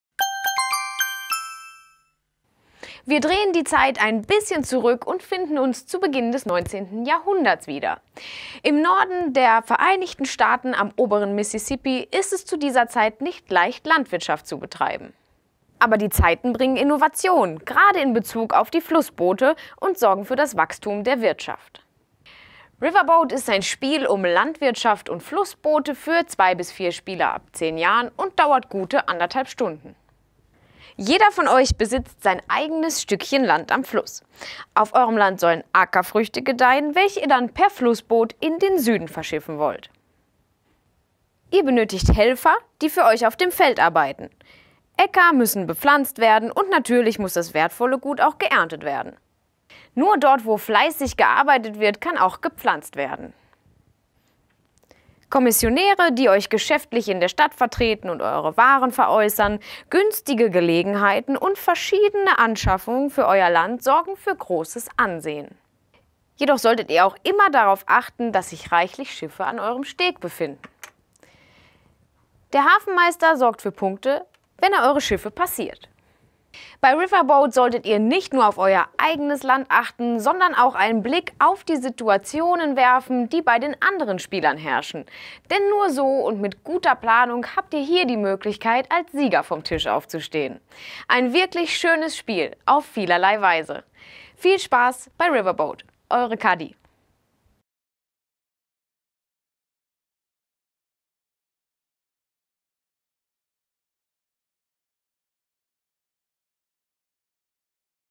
Riverboat | Kurzvorstellung
Riverboat von Michael Kiesling versetzt euch in das 19. Jahrhundert auf eine Farm im nördlichen Amerika. Auf dieser sorgt ihr mit euren Arbeitern dafür, dass die Felder mit unterschiedlichen Früchten bestellt und abgeerntet werden. Im Anschluss werden die geernteten Früchte auf die Namensgebenden „Riverboats“ (engl. Flussboote) verladen und mit ihnen nach New Orleans verschifft.